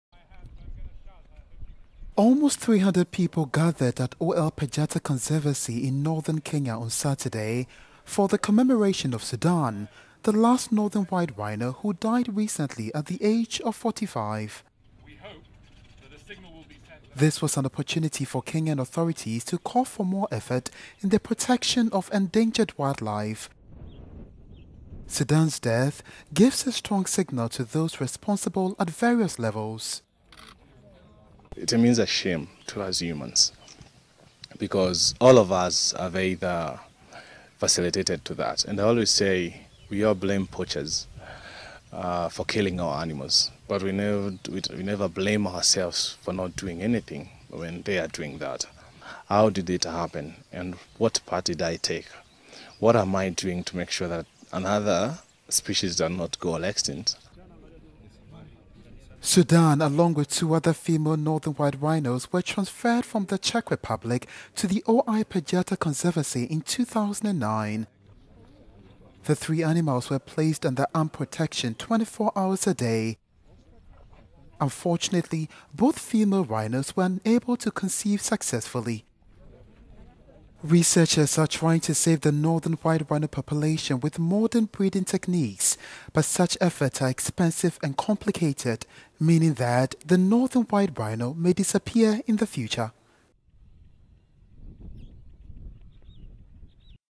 Kenya commemorates last male northern white rhino
Around 300 people gathered in the Ol Pejeta Conservancy in the north of Kenya on Saturday to commemorate Sudan, the last male northern white rhino who recently died aged 45.

Among the attendees was Najib Balala, Cabinet Secretary from Kenya's Ministry of Tourism, which organized the event. Balala expressed in his speech that wildlife is a valuable asset to Kenya and the death of Sudan acts as a reminder that more efforts must be made to protect endangered wildlife.